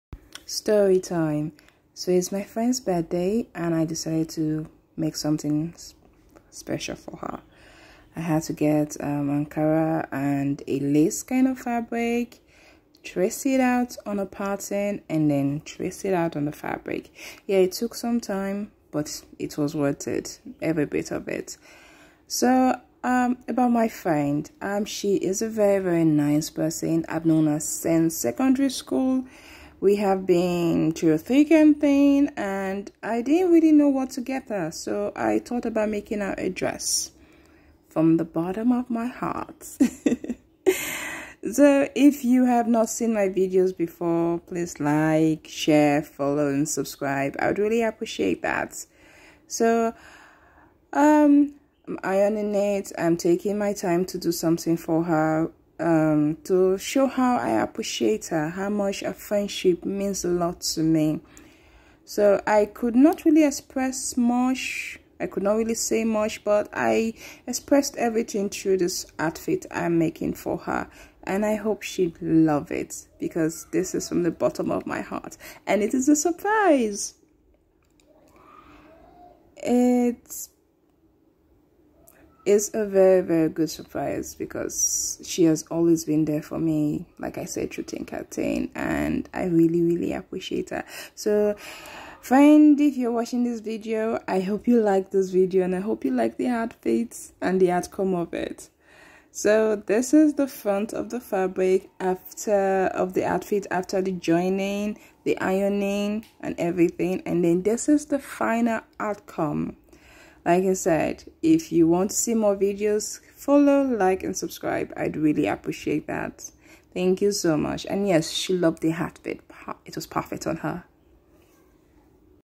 Birthday surprise
join @sokondi couture  in creating a dress from scratch for her friends suprise birthday.